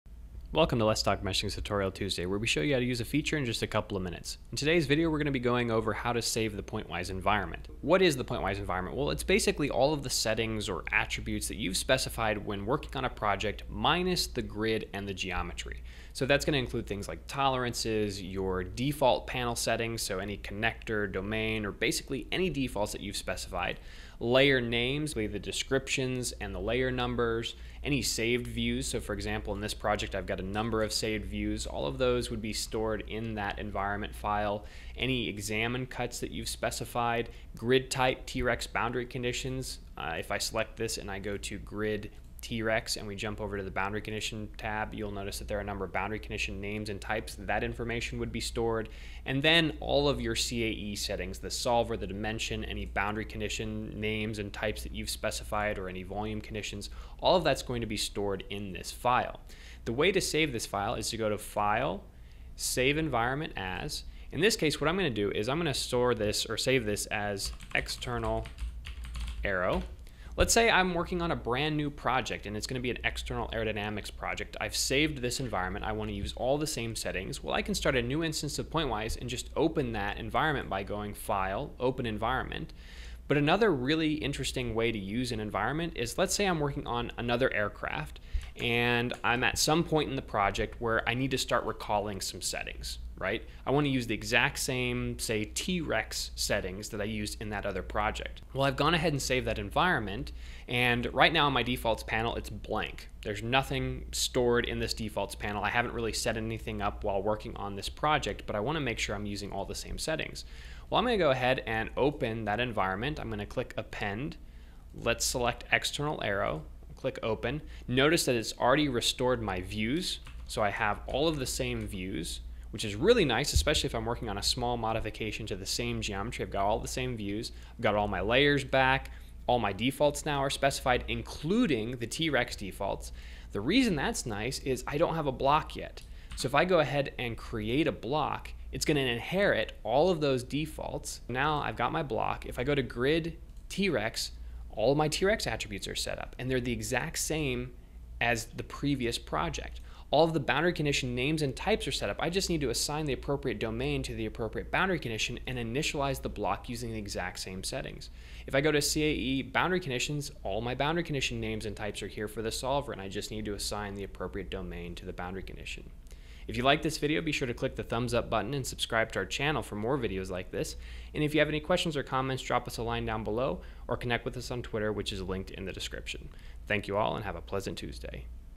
Saving the Pointwise Environment
In this video we demonstrate how to save the Pointwise environment. The Pointwise environment file contains a number of settings and attributes while excluding the grid and geometry. Saving an environment file can accelerate the meshing process for future projects that use the same settings.